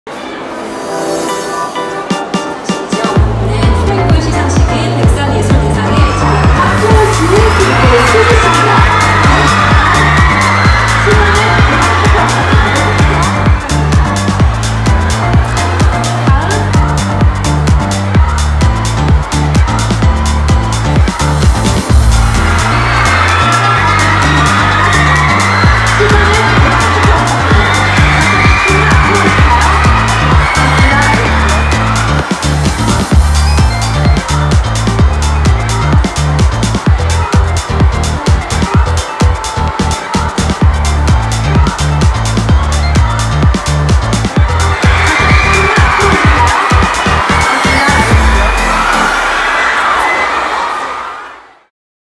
[InstyleKoreaTV] 백상예술대상 비하인드 영상 대공개!
박보검, 류준열, 수지, 윤아 등 지금 가장 핫한 스타들이 참석한 제 53회 백상예술대상의 레드 카펫 현장을 공개합니다.